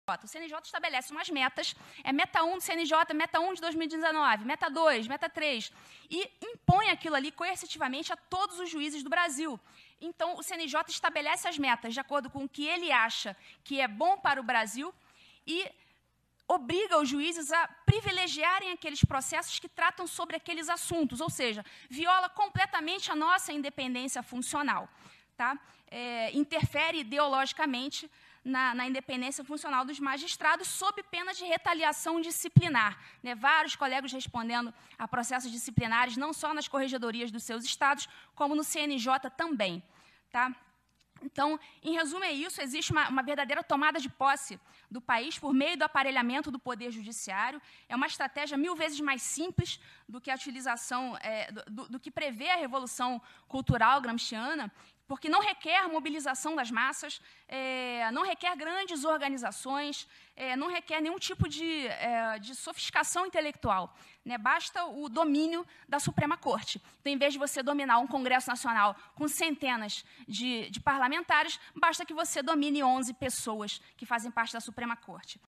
Dra Ludimila Lins Grilo, aparelhamento e domínio da Suprema Corte.
A Dra Ludimila Lins Grilo, Juíza do Tribunal de Justiça de MG, afirma que a tomada de posse do país pela esquerda socialista ocorre por meio do aparelhamento e domínio da Suprema Corte.